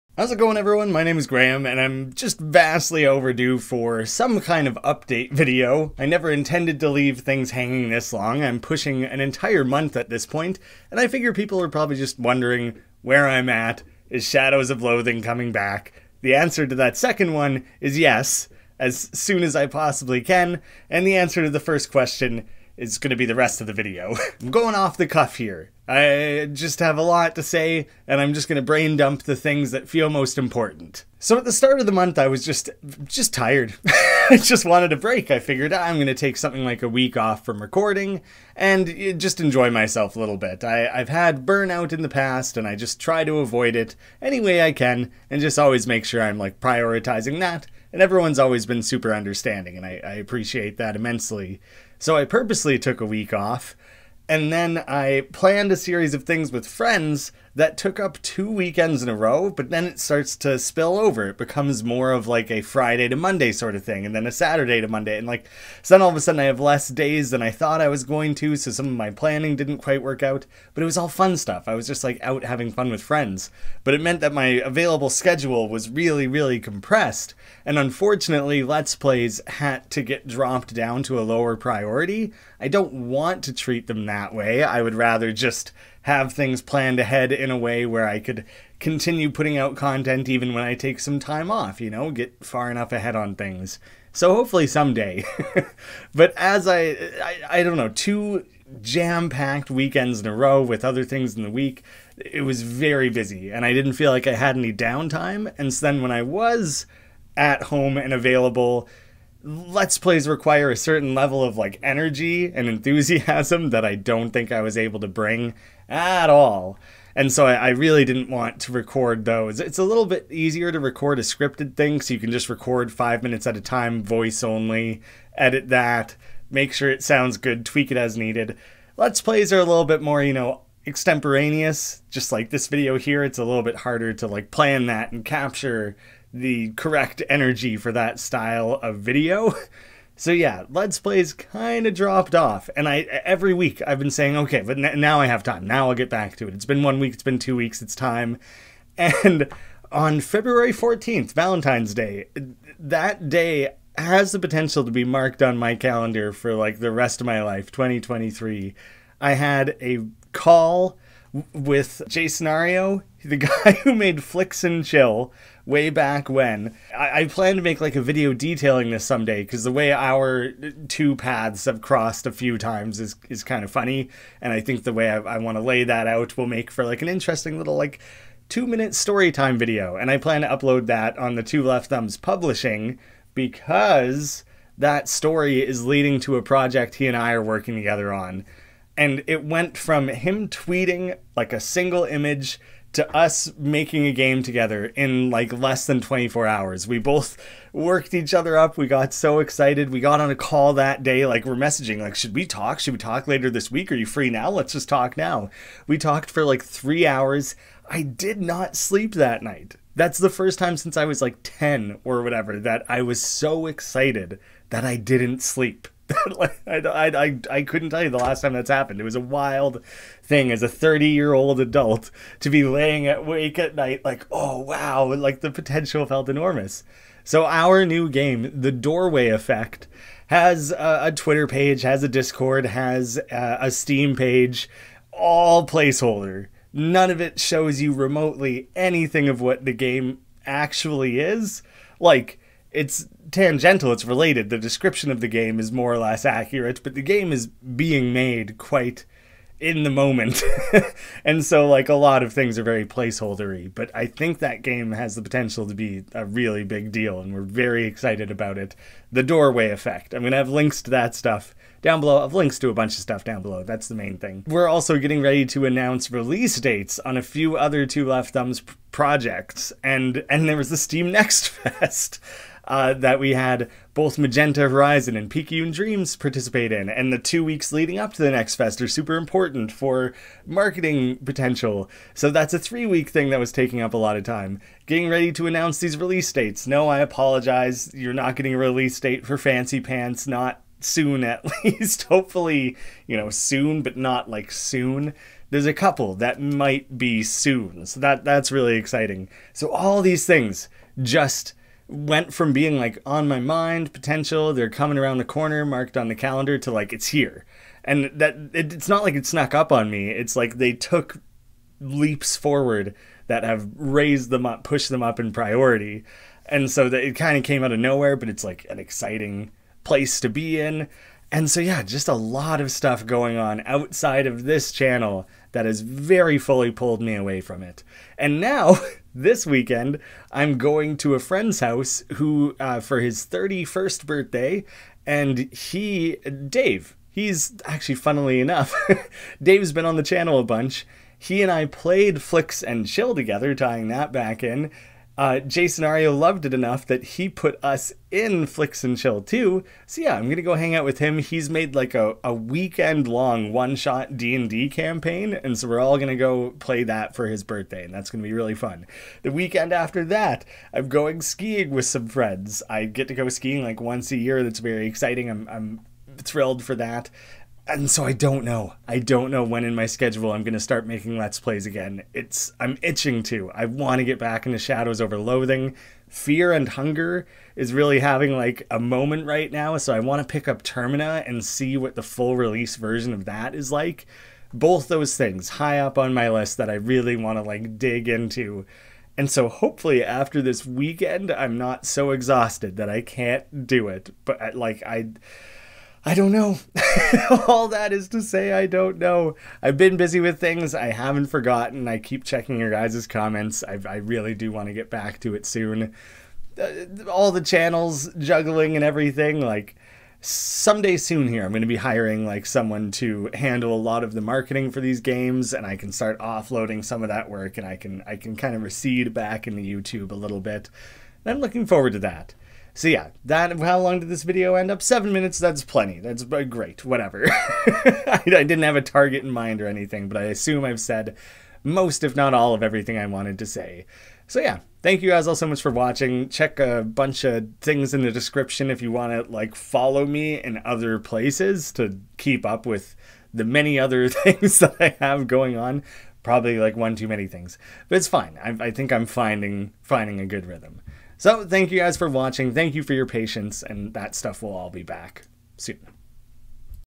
Where have I been?? (February 2023 Update)
I never meant to leave Shadows Over Loathing hanging like this, and I promise I'll get back to it ASAP! Hopefully I can pick up and play some Fear & Hunger: Termina as well :)